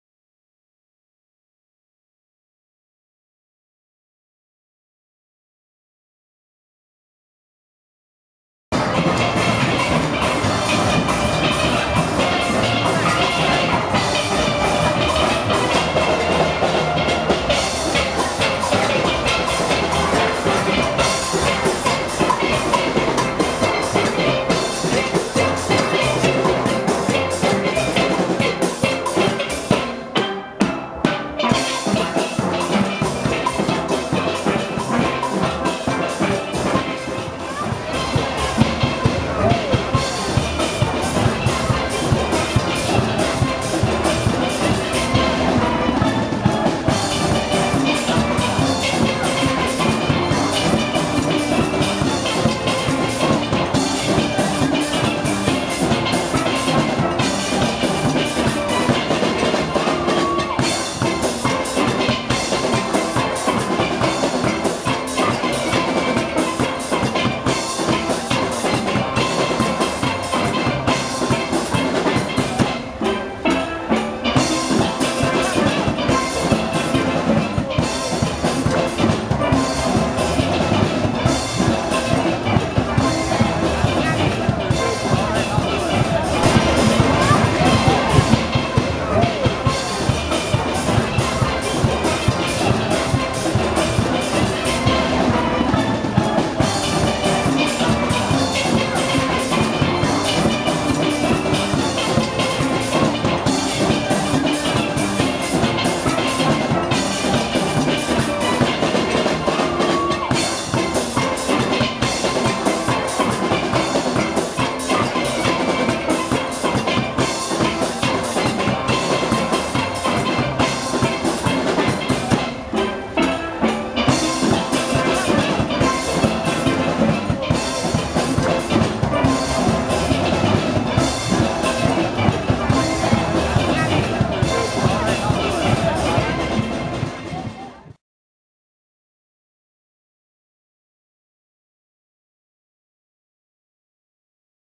Coventry Godiva Carnival 2010 Part 3
Highlights from the procession as it made its way along Kenilworth Road toward the War Memorial Park.